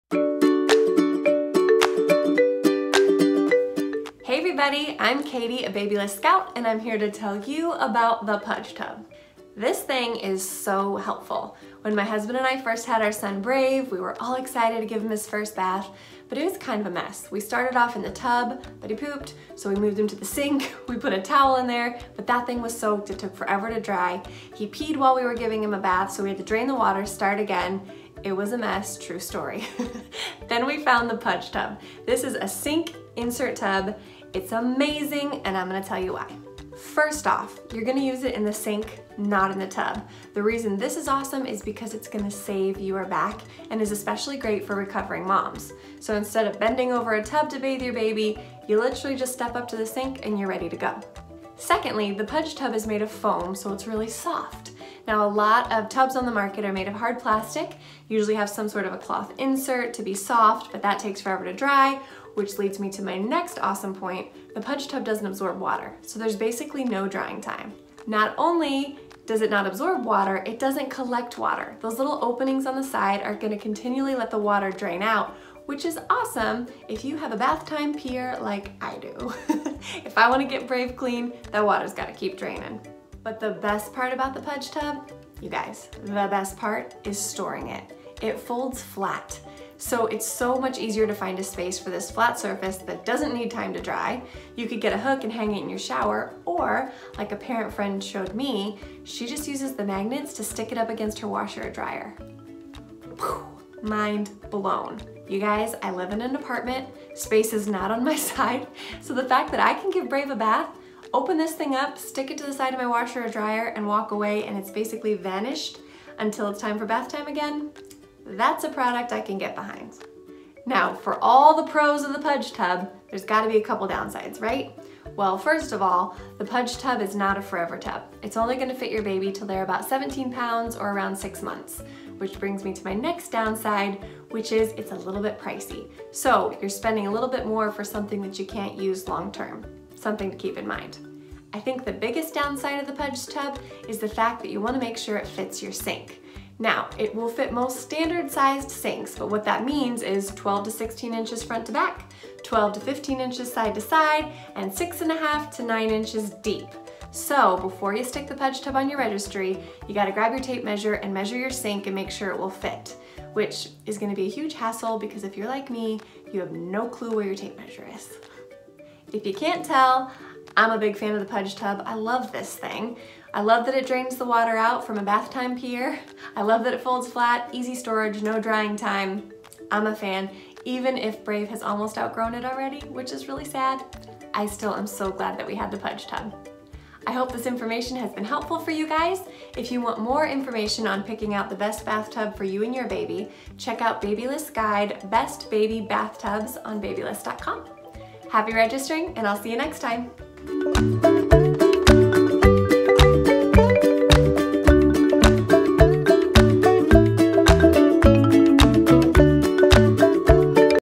Puj Tub Review - Babylist
The Puj tub is truly like no other. The innovative design lets you bathe newborns and young babies confidently in the bathroom sink. Here's Babylist scout Katy to show you more!